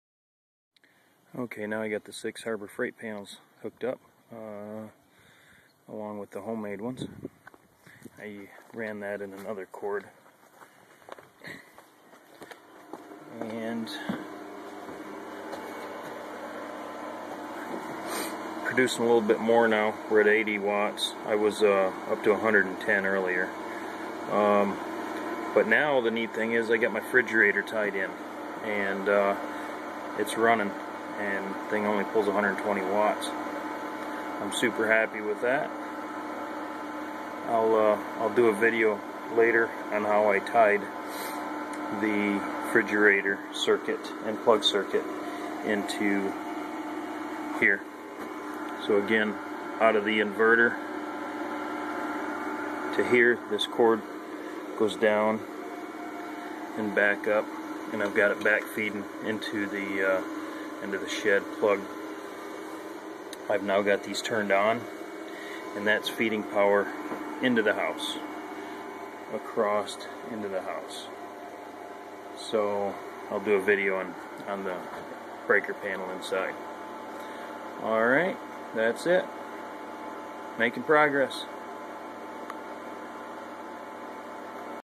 Added the harbor freight solar panels and now running the frig off grid
encompassingselfreliance,com